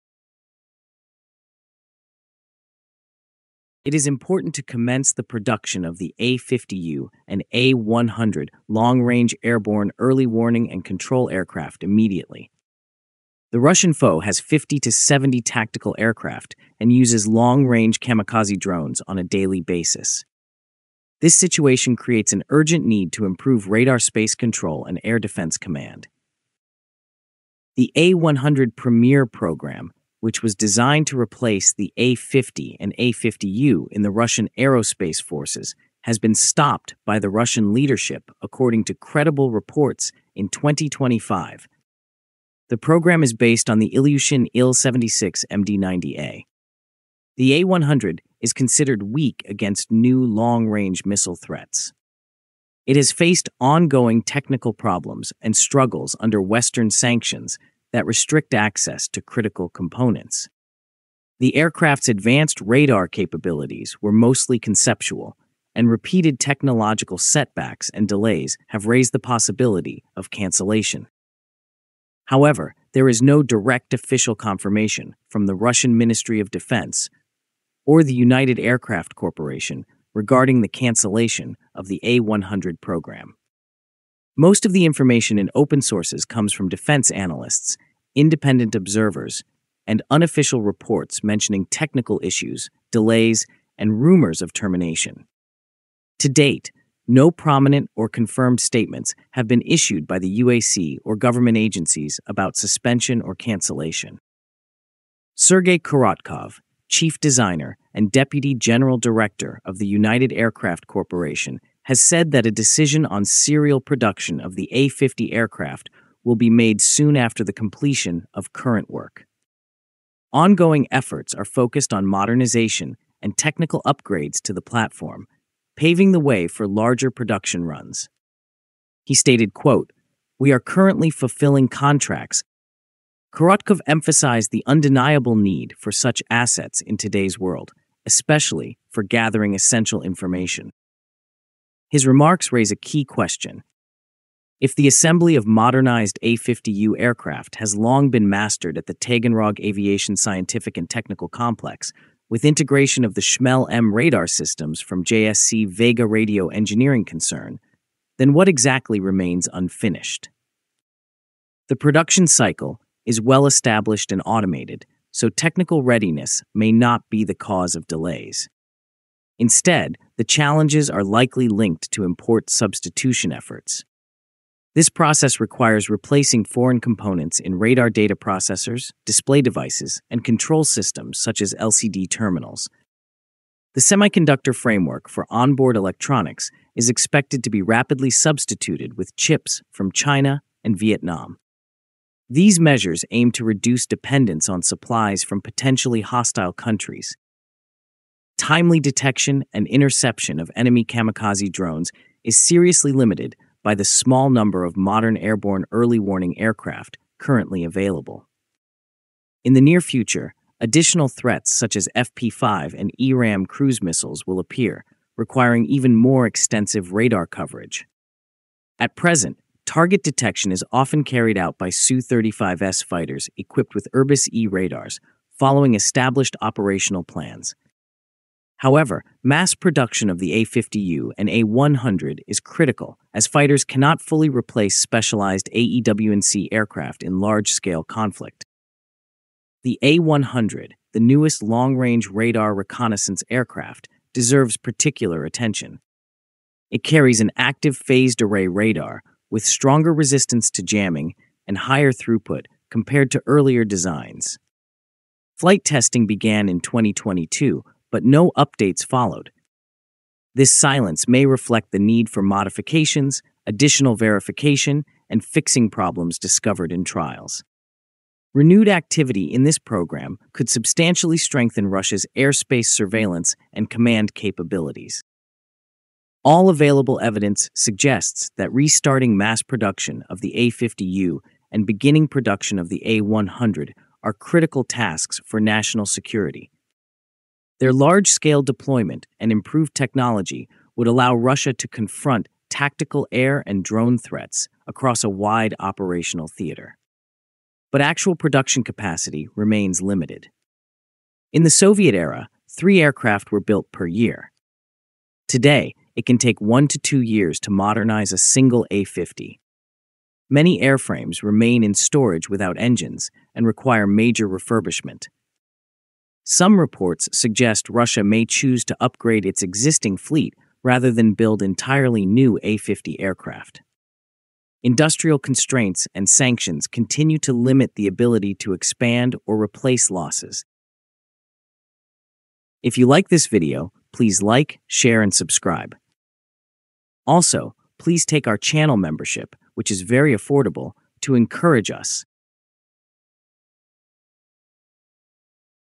Russia Might Restart production of its A-50U AEW&C | Il-76MD-90A | A-100
Russia faces urgent pressure to restart production of its A-50U and A-100 AEW&C (Airborne Early Warning and Control) aircraft as kamikaze drone and missile threats intensify.

The A-100 Premier program, based on the Il-76MD-90A, was supposed to replace the aging A-50 fleet. But reports in 2025 suggest the program has stalled—hit by sanctions, technical issues, and possible cancellation. Despite this, no official confirmation has been given by the Russian Ministry of Defense or United Aircraft Corporation.

In this video, we explore:

Why AEW&C aircraft are vital against drones and cruise missiles

The challenges Russia faces in mass-producing the A-50U and A-100

The role of sanctions and import substitution in slowing development

What the future holds for Russia’s airborne radar capabilities